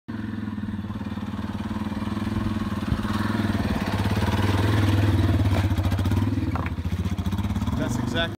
March 2017 ADV skills training Moab - 100
Dennis going over makeshift rock ramp. Adventure Riding. Dual Sport video